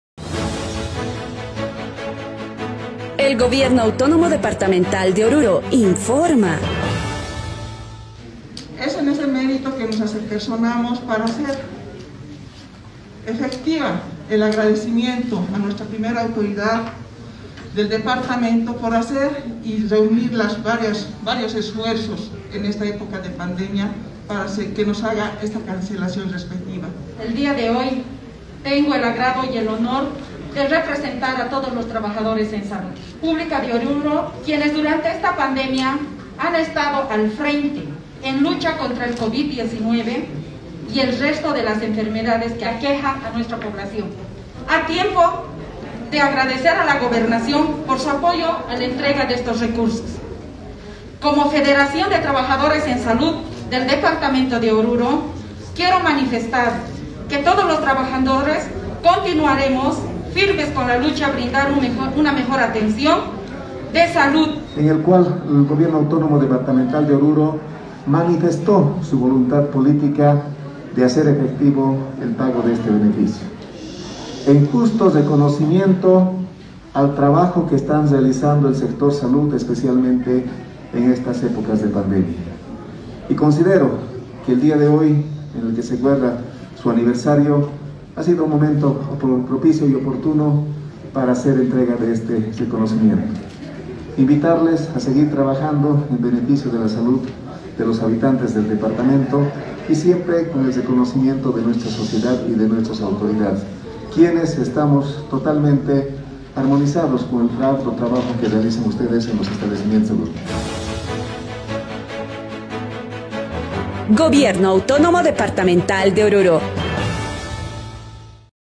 EN MÉRITO AL TRABAJO DESEMPEÑADO EN LA PANDEMIA EL GOBERNADOR ENTREGÓ CHEQUE A TRABAJADORES EN SALUD
El Gobernador Johnny Vedia Rodríguez en el salón Ildefonso Murguía de la Gobernación en el día del trabajador en salud  hizo la entrega de un cheque por más de 6 millones de bolivianos por pago de viáticos de vacunación y escalafón por años de servicio a trabajadores en salud de la gestión 2021.
“Como un justo reconocimiento se nos hace el pago del bono al trabajador por aquello quiero hacer efectivo el agradecimiento a nuestra primera autoridad de departamento por no escatimar esfuerzos en esta época de pandemia”, agradeció a nombre de los trabajadores  en salud pública la Dr. Ayde García D. Representante de la Confederación de Salud  Pública Nacional.